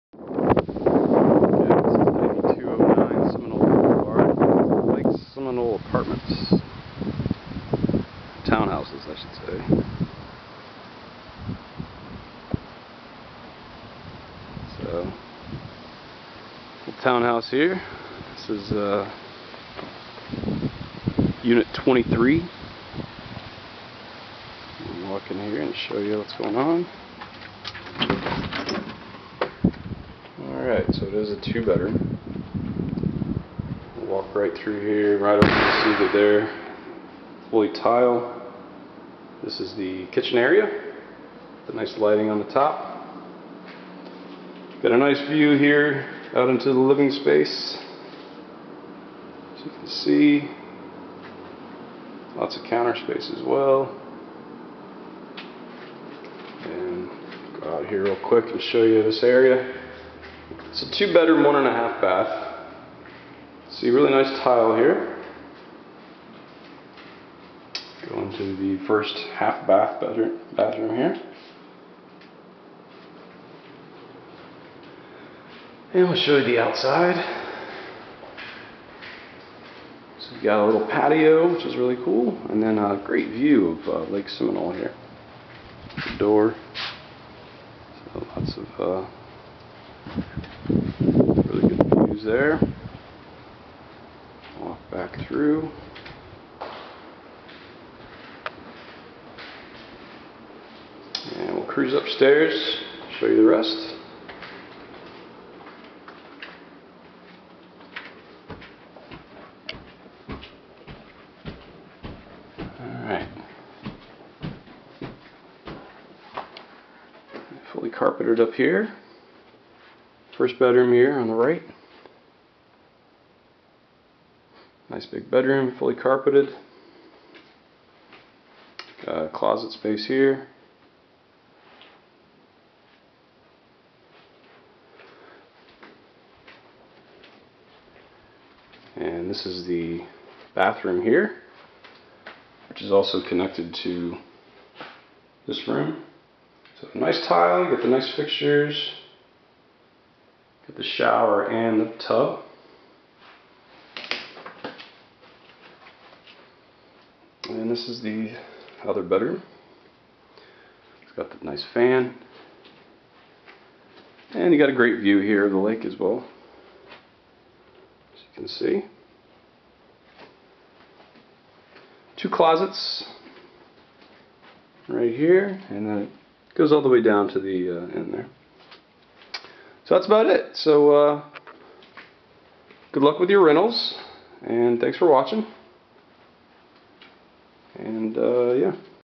Seminole: 1015 sq. ft. 2/1 Townhouse at 9209 Seminole Blvd Unit #23
LOCATION, LOCATION, LOCATION! DON'T MISS THIS FANTASTIC WATERFRONT 2BR/1 1/2 BATH TOWNHOME IN SEMINOLE! TOWNHOME BOAST TILE FLOOR ON MAIN LEVEL, DRYER INSIDE UNIT (NO WASHER) LEFT FOR CONVENIENCE OF TENANT, CEILING FANS, UPDATED BATHS, COVERED PARKING AND SCREENED PATIO WITH SPECTACULAR WATERVIEWS TO ENJOY WILDLIFE. BASIC CABLE, LAWN MAINTENANCE, POOL MAINTENANCE, EXTERIOR BUILDING, WATER, SEWER, TRASH AND BASIC CABLE INCLUDED! FIRST AND SAME SECURITY APPLY. SMALL PET OK AT OWNER'S DISCRETION. ASSOCIATION FEE IS $50. SEMINOLE ELE, OSCEOLA MID AND SEMINOLE HIGH ZONED. TOWNHOME IS CLOSE TO SHOPPING, FINE DINING, FAMILY ENTERTAINMENT, GROCERIES, BANKS AND ONLY MINUTES TO THE WHITE SANDY BEACHES OF PINELLAS COUNTY! FIRST AND SAME BEING SECURITY. $50 APPLICATION FEE PER ADULT. $300NR PET FEE IF APPLICABLE.
9209 SEMINOLE BLVD UNIT #23, SEMINOLE, FL 33722. NEED PROPERTY MANAGEMENT?